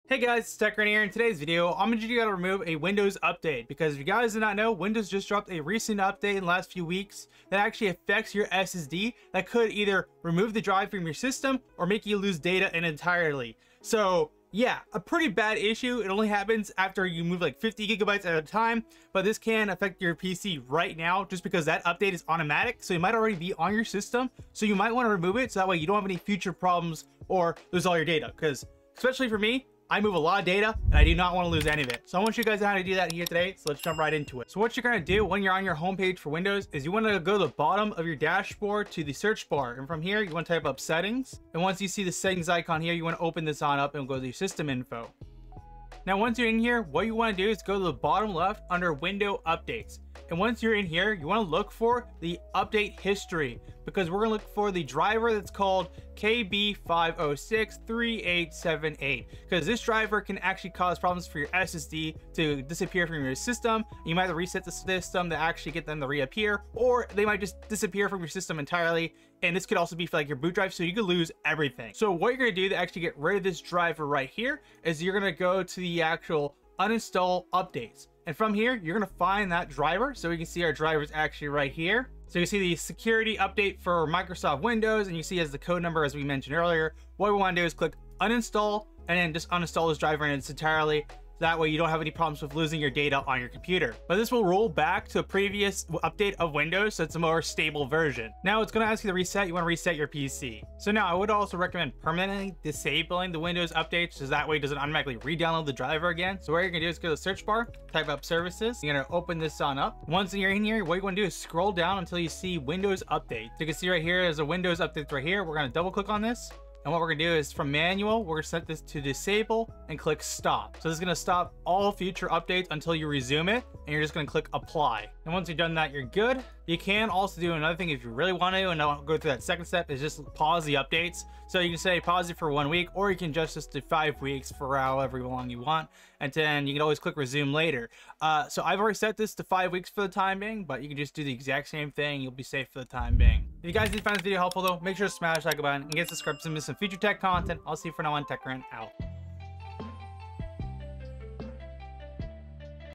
This Windows Update can Destroy your SSD! Watch Right Now
Today I will teach you how to uninstall a recent windows update that can cause major problems for SSDs. I would show you how to uninstall windows 11 updates.

📦 All purchases for yourself will directly support my YouTube at no cost!